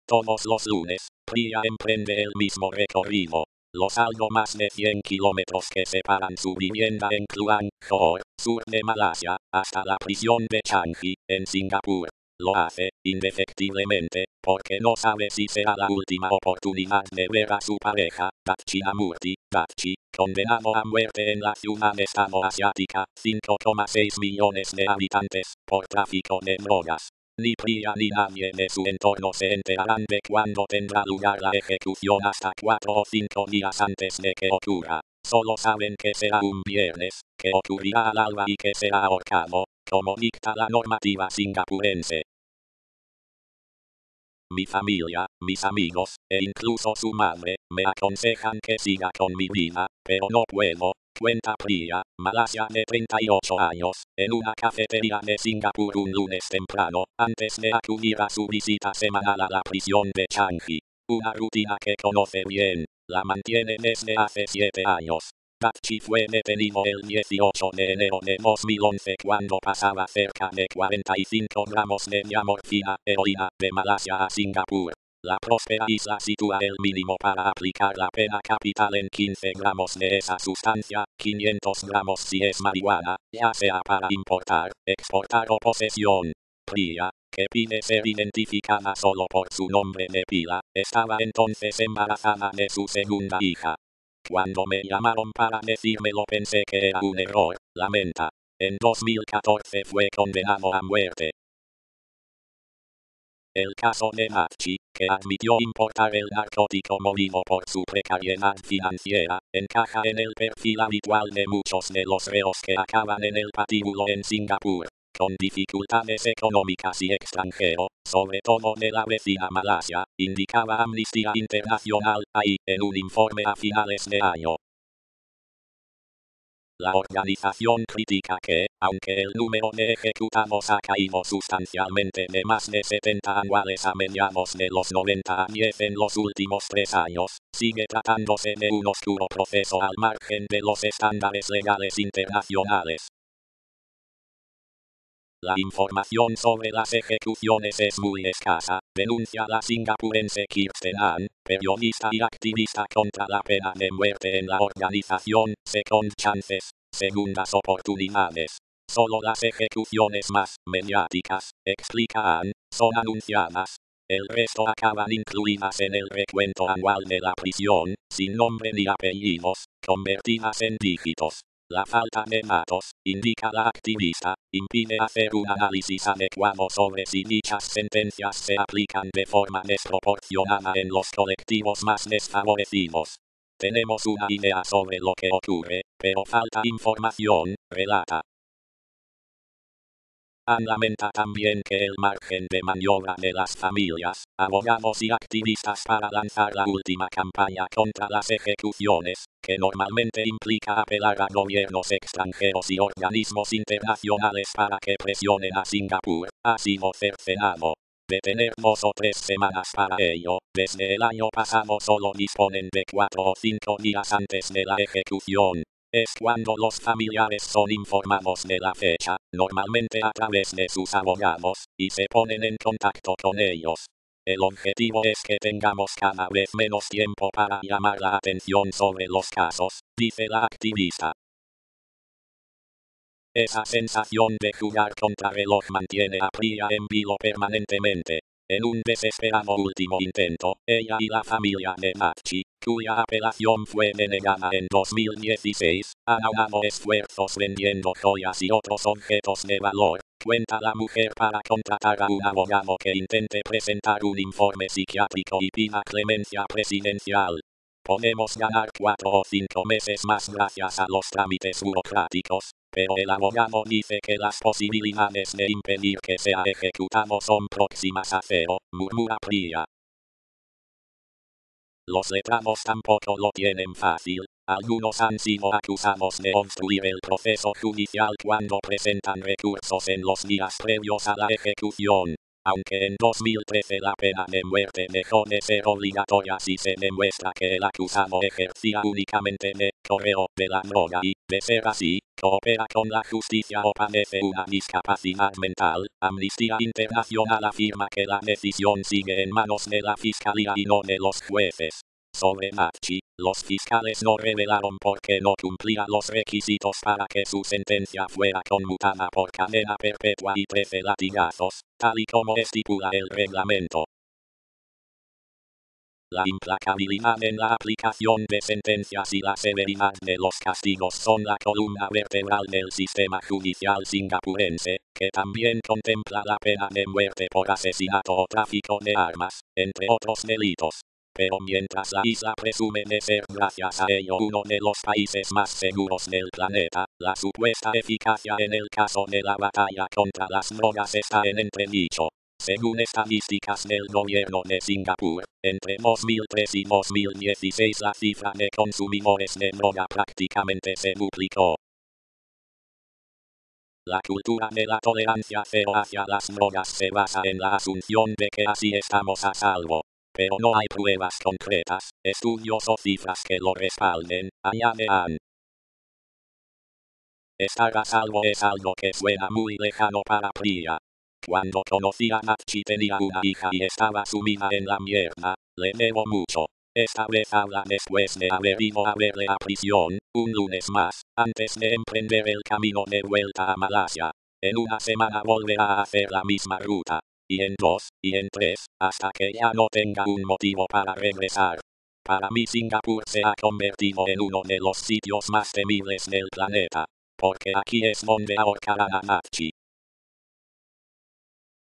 Singapur: a la horca por narcotráfico (con solo cinco días de aviso)
Todos los lunes, Priya emprende el mismo recorrido. Los algo más de cien kilómetros que separan su vivienda en Kluang (Johor, sur de Malasia), hasta la prisión de Changi, en Singapur. Lo hace, indefectiblemente, porque no sabe si será la última oportunidad de ver a su pareja, Datchinamurthy, Datchi, condenado a muerte en la ciudad-estado asiática (5,6 millones de habitantes) por tráfico de drogas. Ni Priya ni nadie de su entorno se enterarán de cuándo tendrá lugar la ejecución hasta cuatro o cinco días antes de que ocurra: solo saben que será un viernes, que ocurrirá al alba y que será ahorcado, como dicta la normativa singapurense. V: ves_002339.mp4